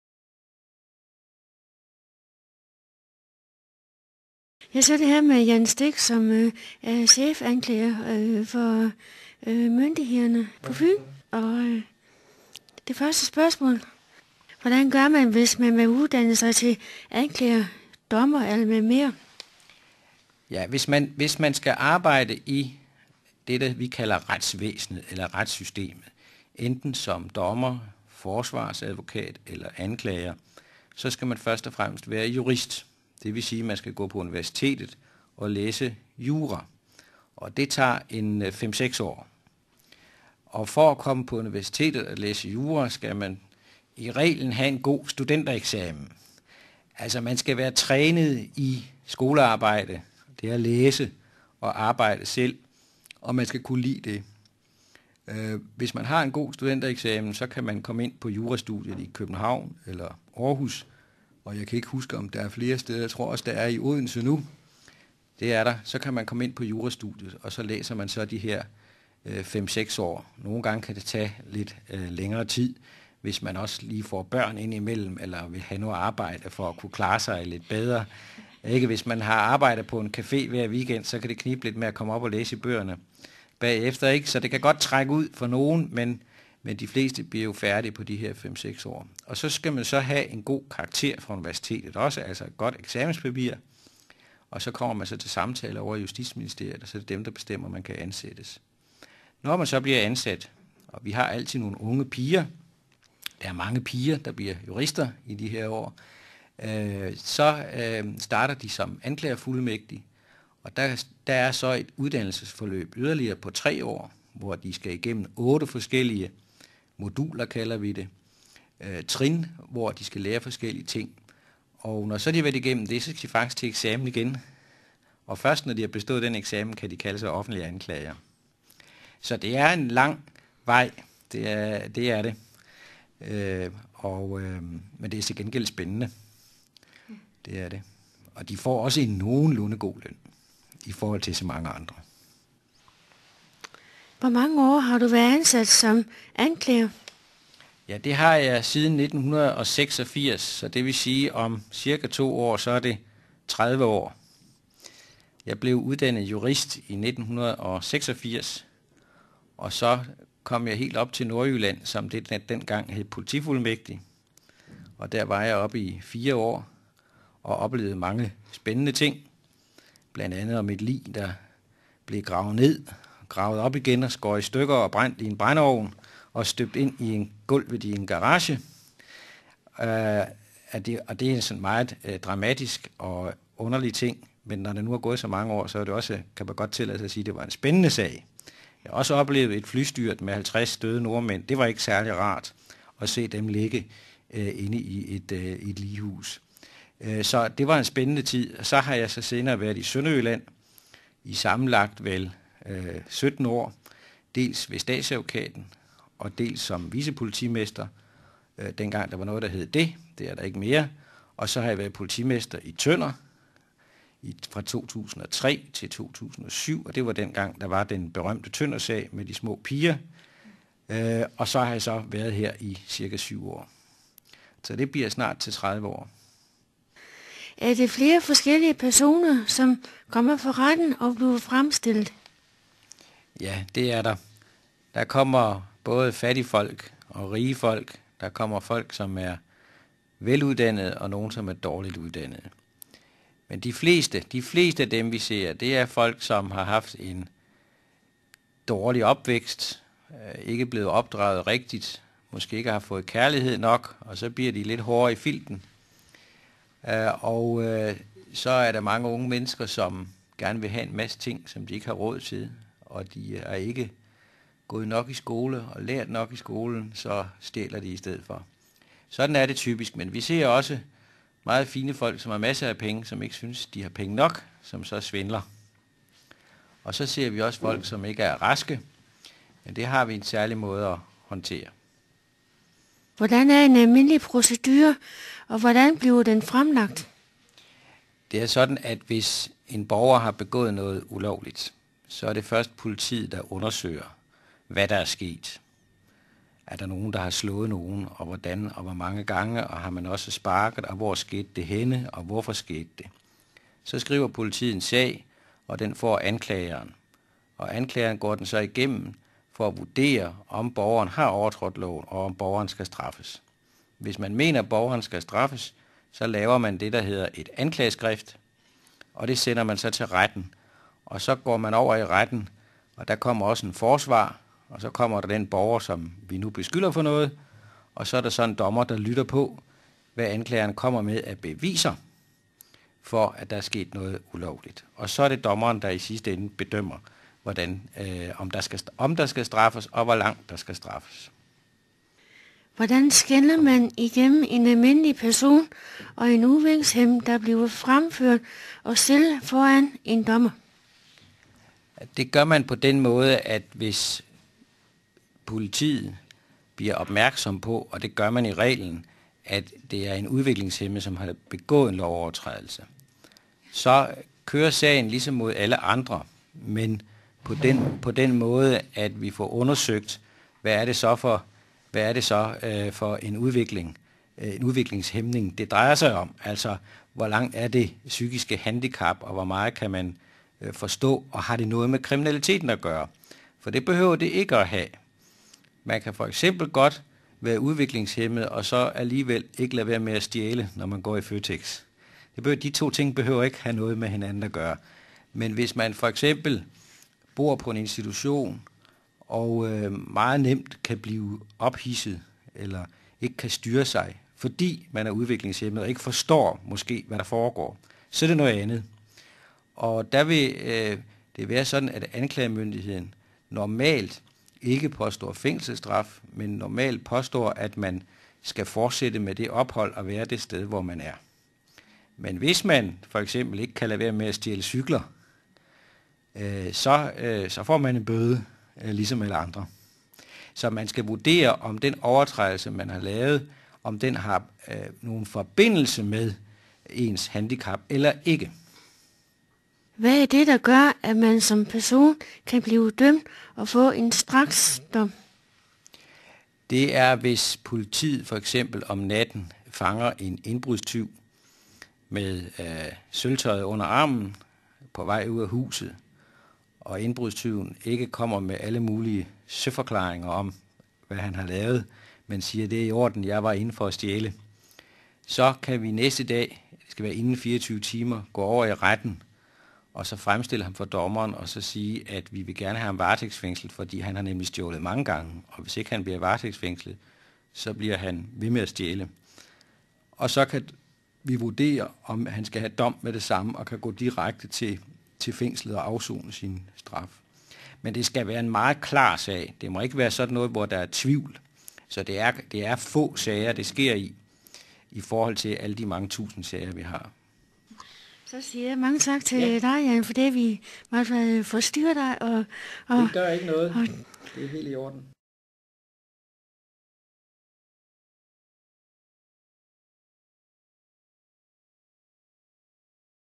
Jan Stick
Kirsten har mødt chefanklager Jan Stick fra Fyns politi